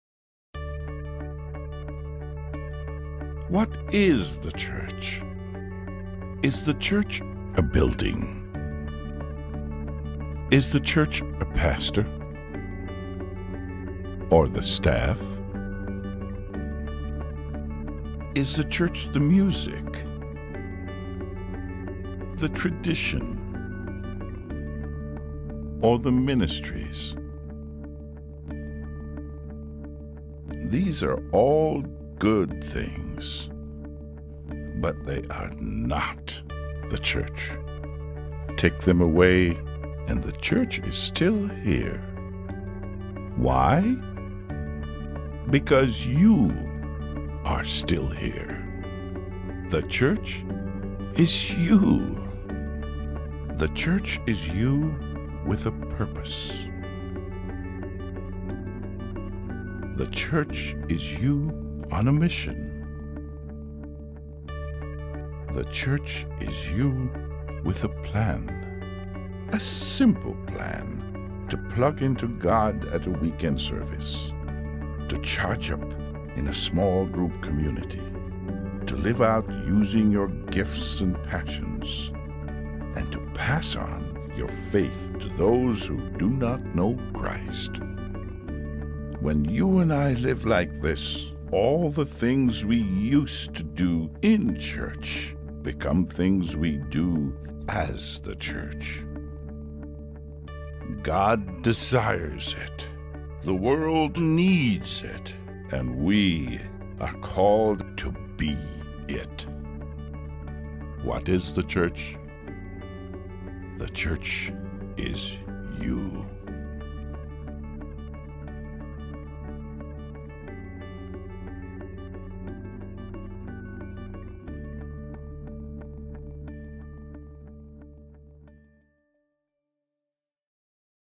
Wie of wat is die kerk? (Engels)
'n Kort sketsbeskrywing oor betrokkenheid, deelname en meelewendheid van lidmate by die kerk. van lidmate